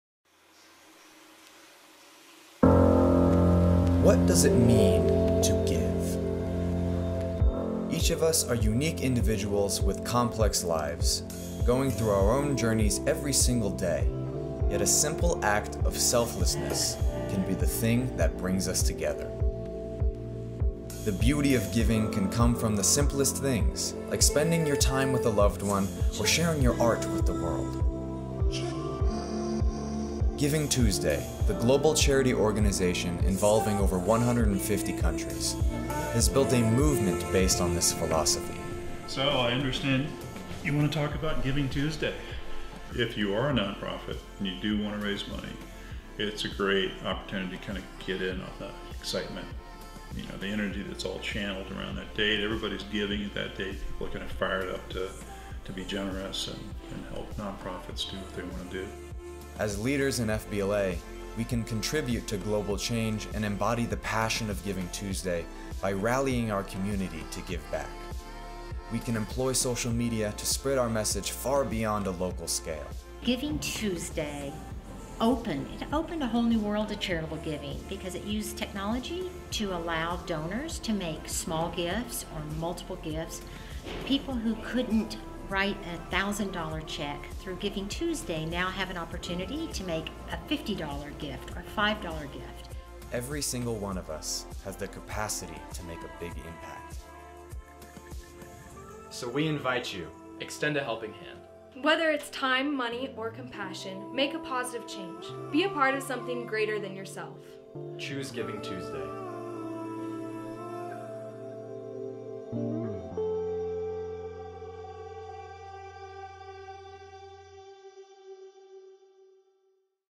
What does it mean to give? - Giving Tuesday [FBLA Digital Video Production 2019]
After earning second place at CODLC19, we present our state submission for FBLA Digital Video Production 2019. Created by Alexandre Shappell, Will Cutchin, and Lena Bodenhamer.

Special thanks to Marshall Cutchin and Nina Bodenhamer.

▶️ Kasbo